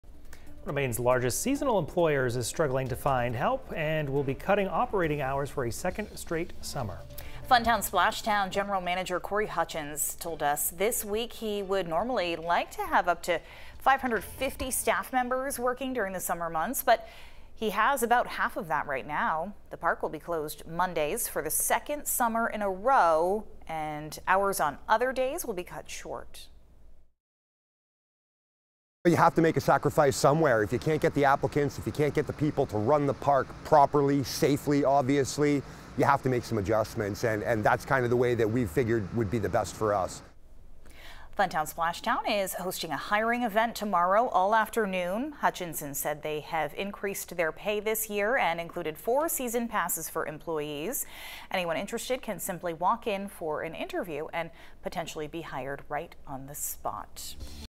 One of Maine's largest seasonal employers cuts hours, struggles to find workers
Funtown Splashtown USA will be closed on Mondays for the second year in a row.